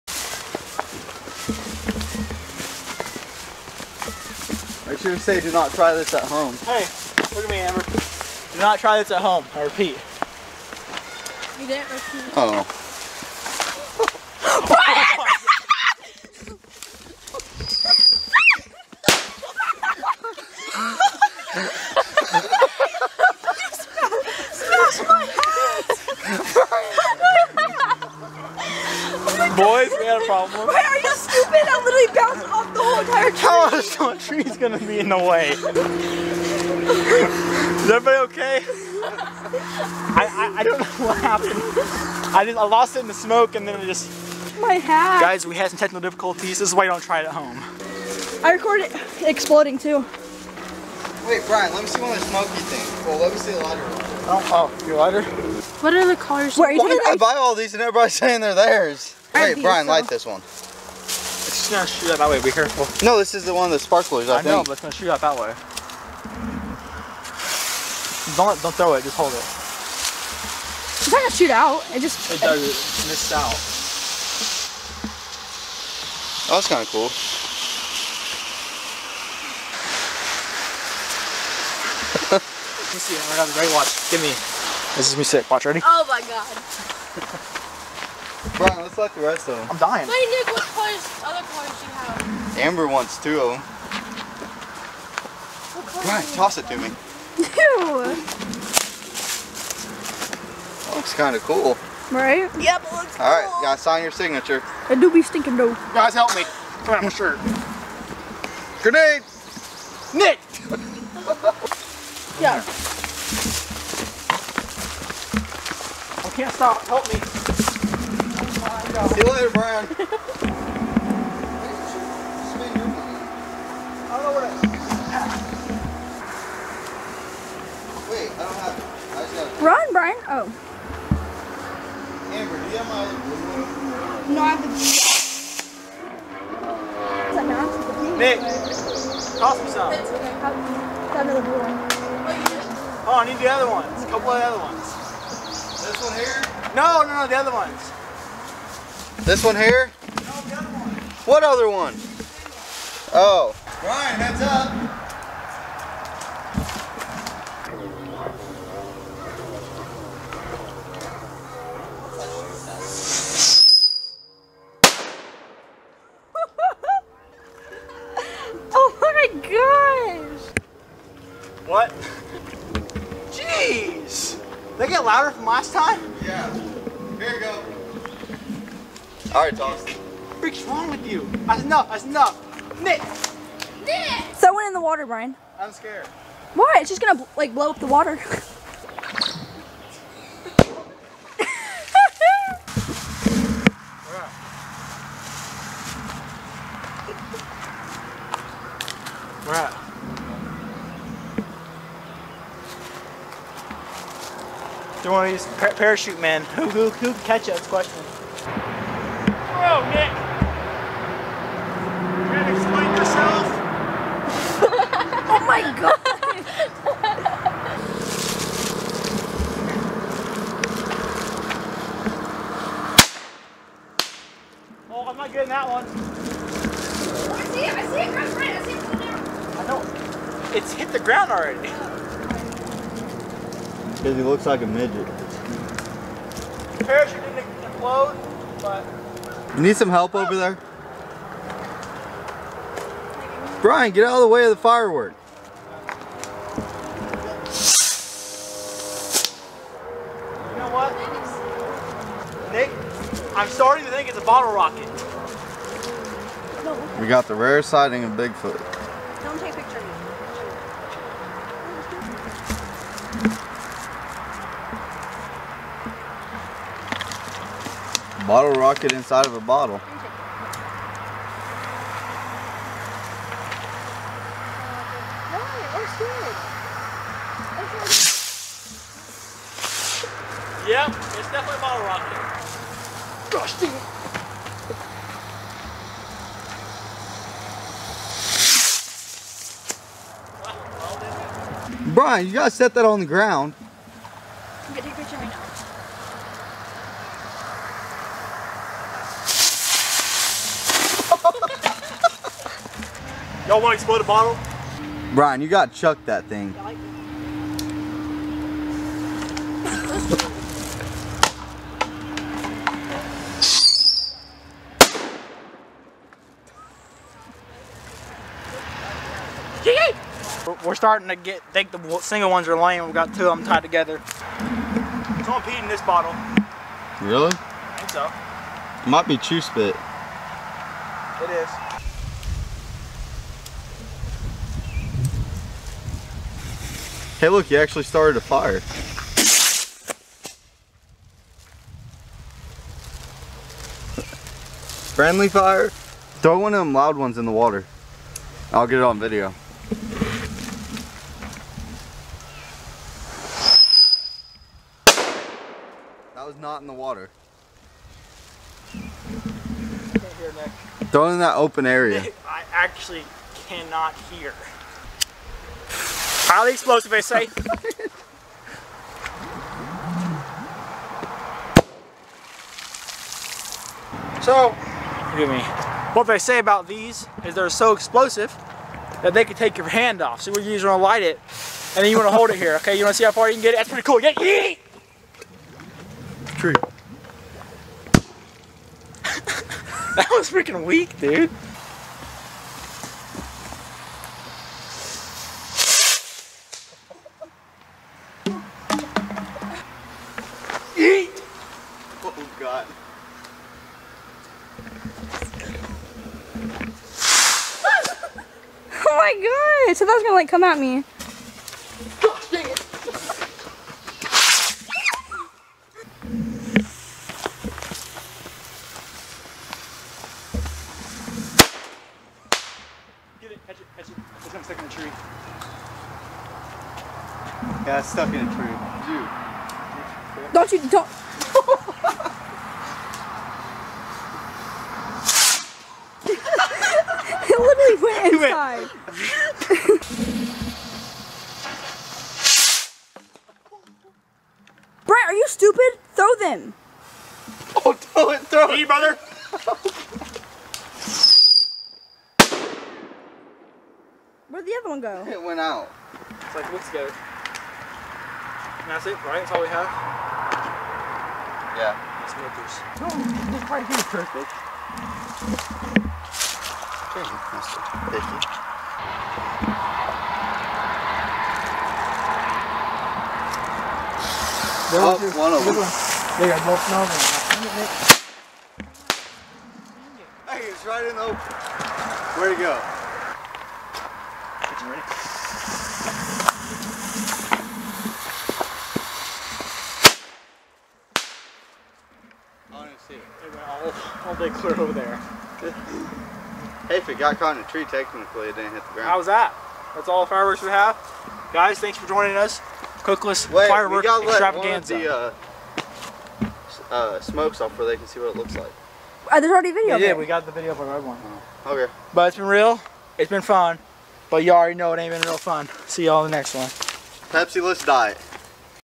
Crazy 4th of July Fireworks!!
Merica day was AWESOME watch us celebrate with friends and fireworks. More content on tiktok: